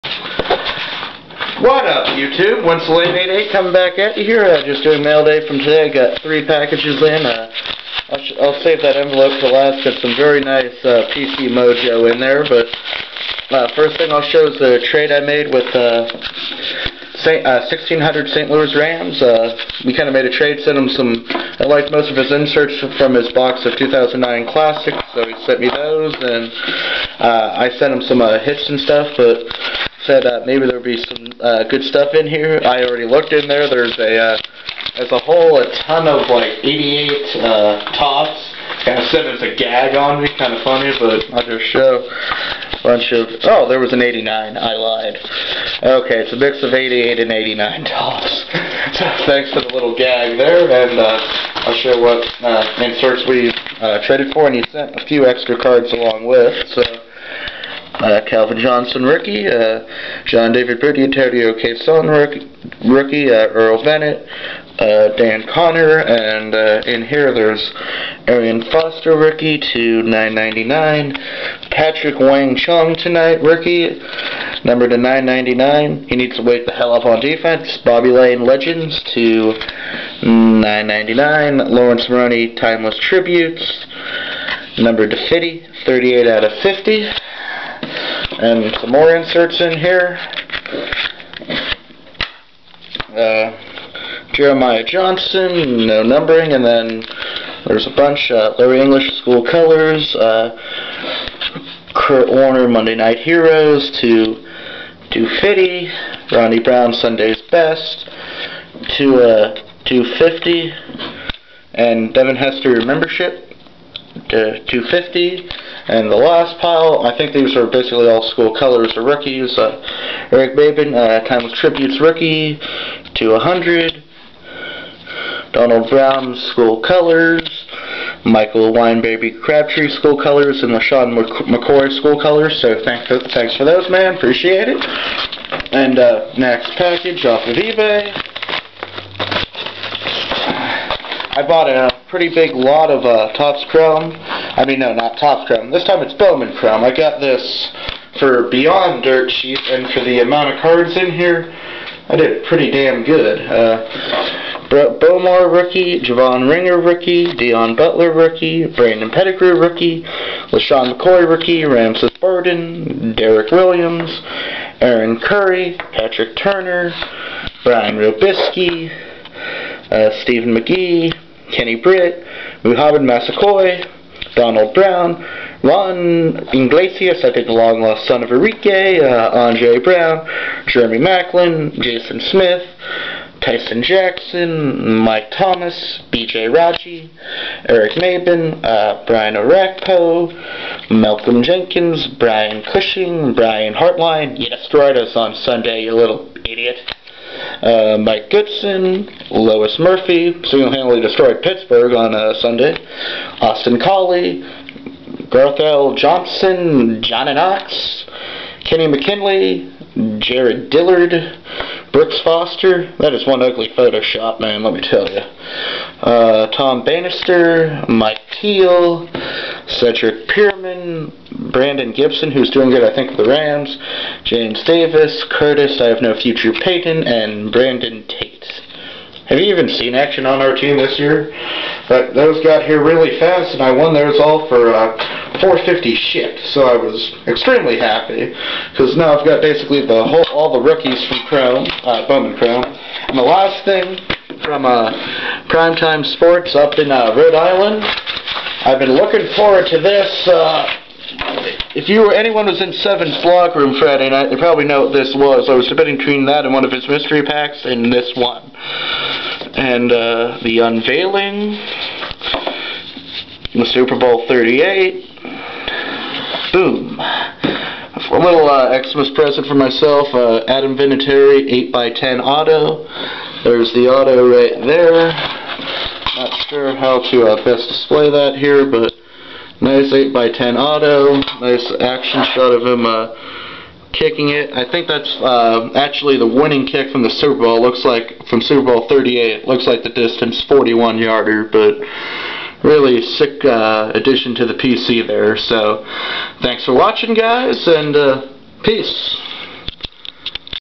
Mailday (1600Stlouisrams, and ebay) Patriots PC Mojo!
Small mailday from today, but got a really sick autographed photo for the PC.  Check out this youtuber: